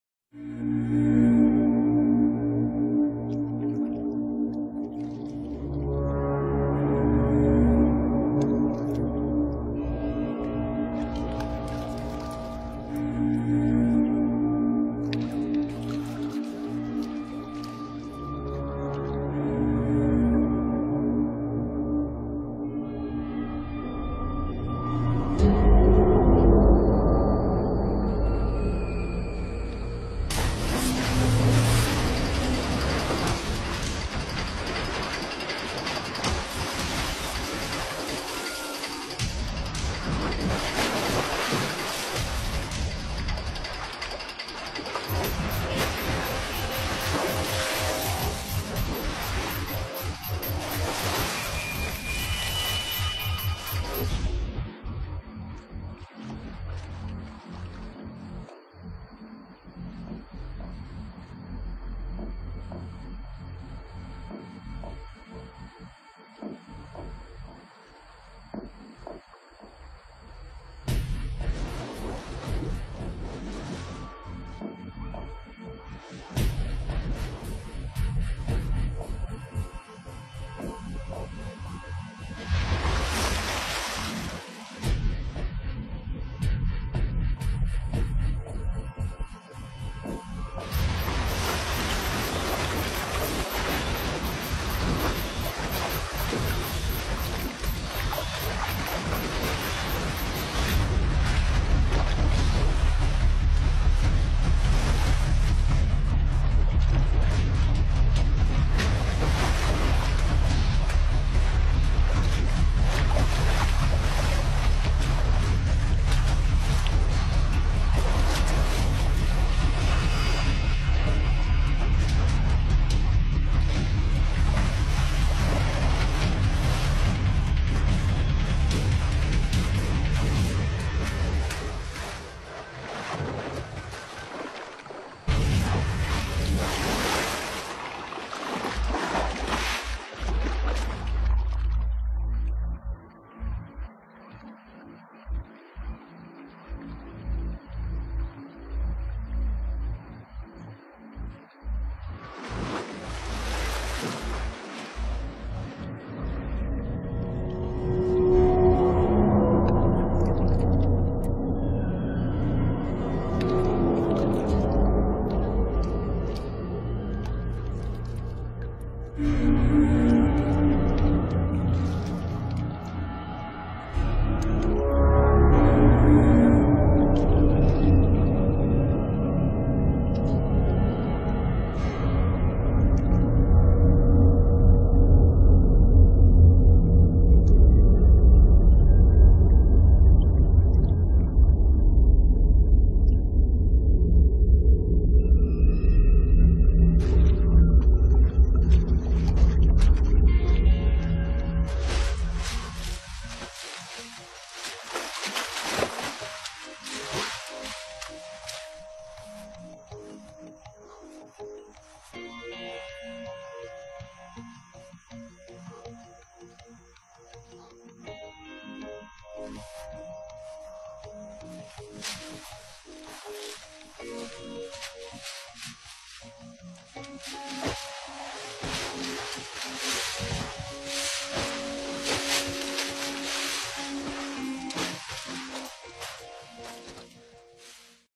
ONÇA PINTADA EM ATAQUE AÉREO MORTAL EM CIMA DE UM GRANDE JACARÉ
Nesse vídeo vc vai assistir um ataque mortal de uma onça pintada em cima de um jacaré !